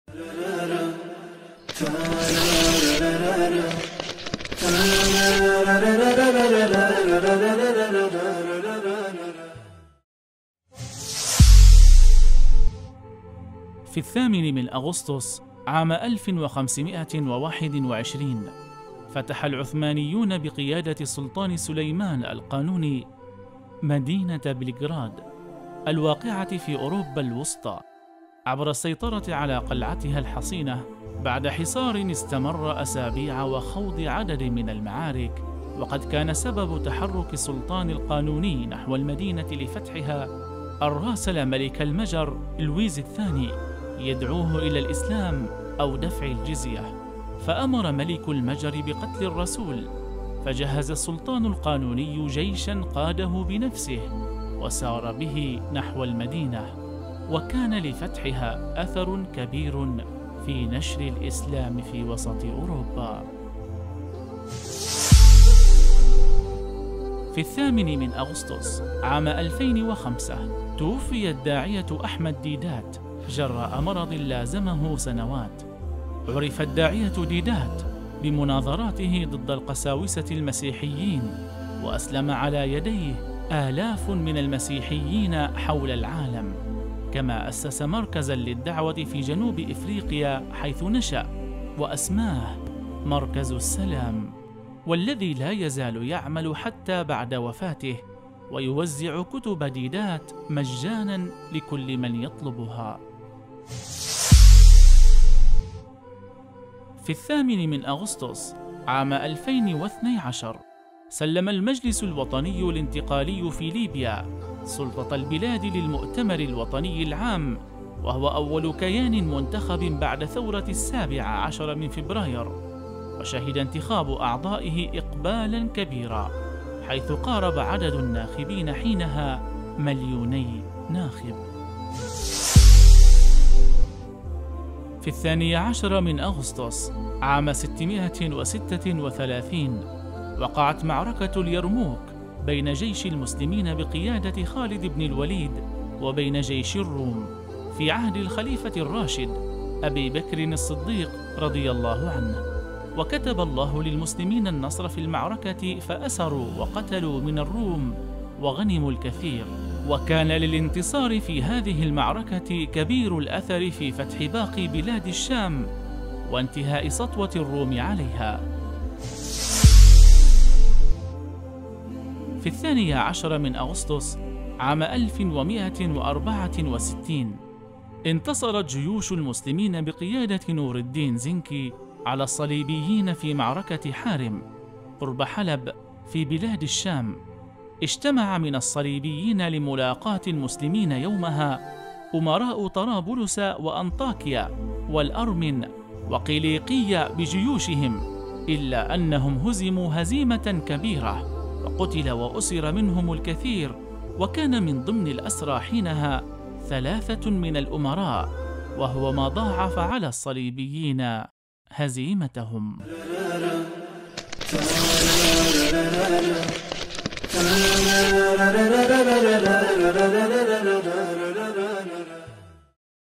وفاة الداعية احمد ديدات، وذكرى وقوع معركة اليرموك بين المسلمين والروم
هذا وأكثر ضمن حلقة تاريخ وحدث

ــــــــــــــــــــــــــــــــــــــــــــــــــــــــــــــــــــــــــ
ــــــــــــــــــــــــــــــــــــــــــــــــــــــــــــــــــــــــــ
يمكنكم تحميل تطبيقنا TANASUH TV على الهواتف الذكية: